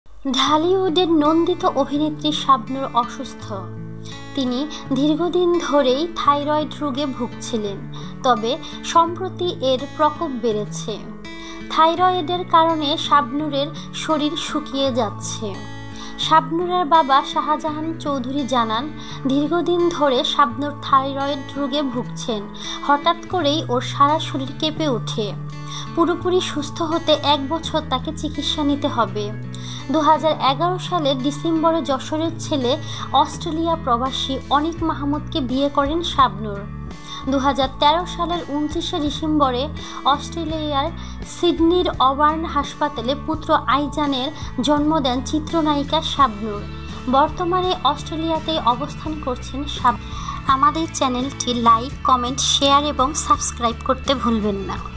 এ কেমন রোগে ভুগছে চিত্র নায়কা শাবনূর !!!Shabnur!!!Latest Bangla news!!!
ঢালিউডের নন্দিত অভিনেত্রী শাবনূর অসুস্থ।শাবনূর থাইরয়েড রোগে ভুগছিলেন।তিনি বর্তমানে অস্ট্রেলিয়াতেই আছেন।তিনি অনেক শুকিয়ে গেছে।শাবনূর দেশ বাসীর কাছে দোয়া চেয়েছেন।